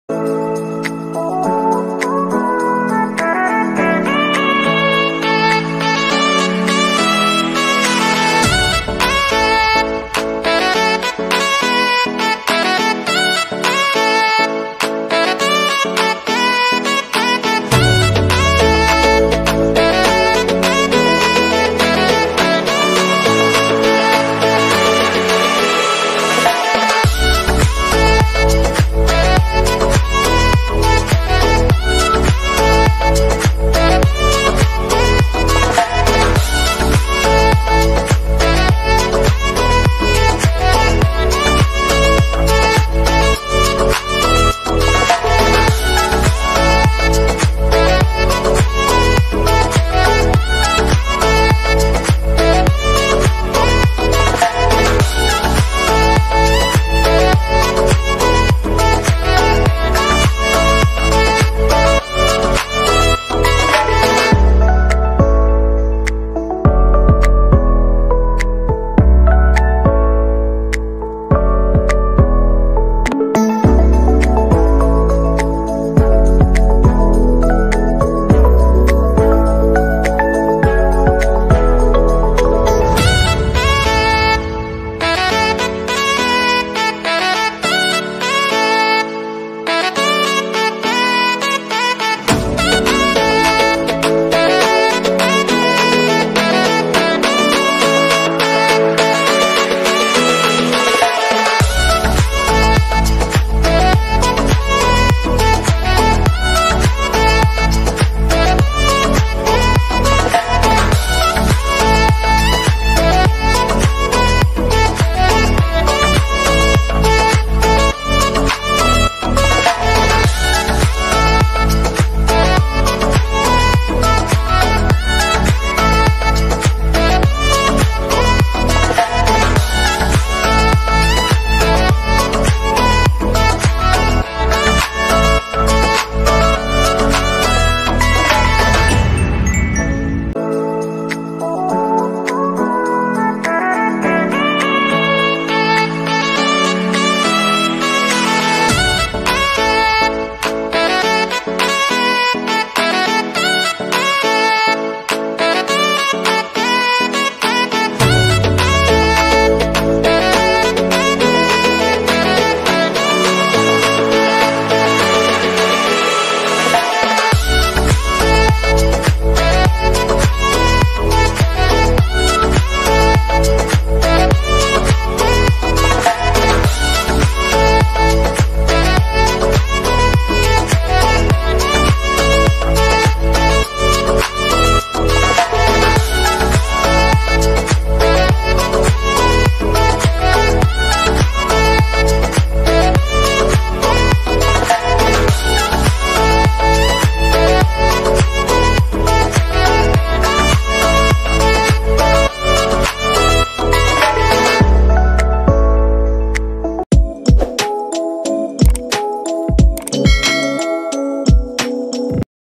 Vitamins Sea Enjoy Ang Mga Bagets
Finally nakapag swimming din ulit after ng ilang months na hindi makalabas.Open na ulit